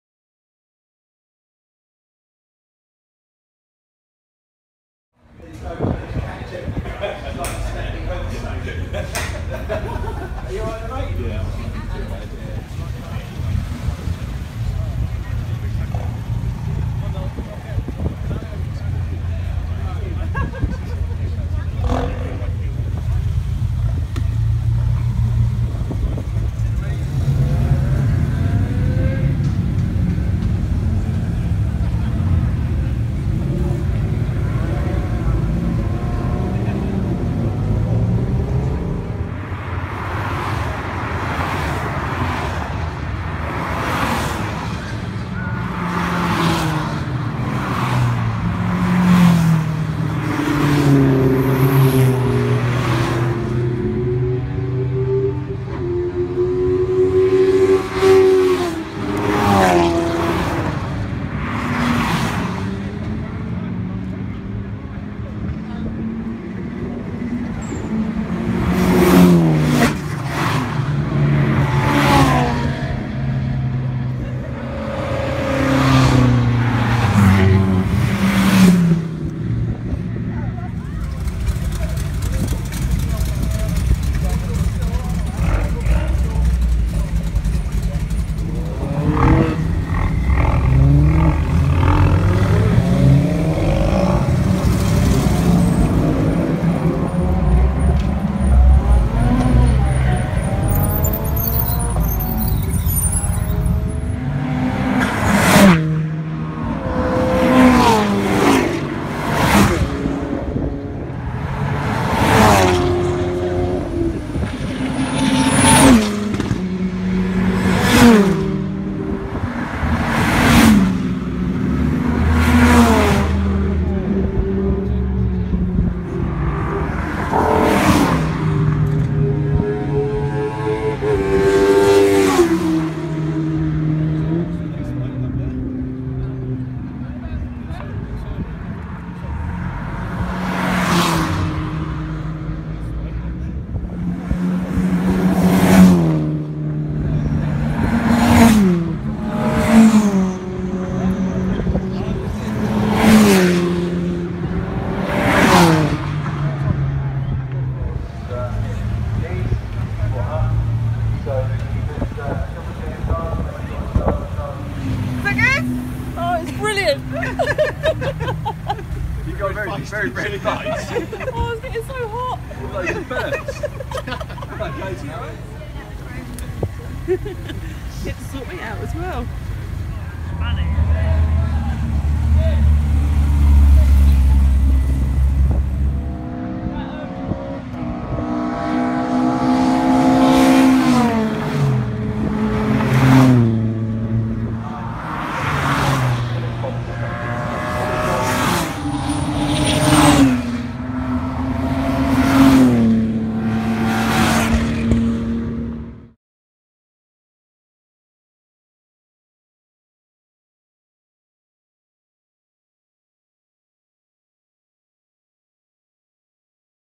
Pro Alloy Track Event in Memory of our late Co-founder Wayne Clayden
Just as Wayne wanted, "you'd better do something good!"
Well, we hope we did you proud Wayne.
We had a fab night, thank you all for coming to celebrate a life taken too soon, a life we will never forget, Our amazing thewheelieking Hero.
The Macmillan Nurses will also be receiving £262, which is just brilliant! well done and thank you to each and everyone of you.
Same again next year? 🦄💙👍🏽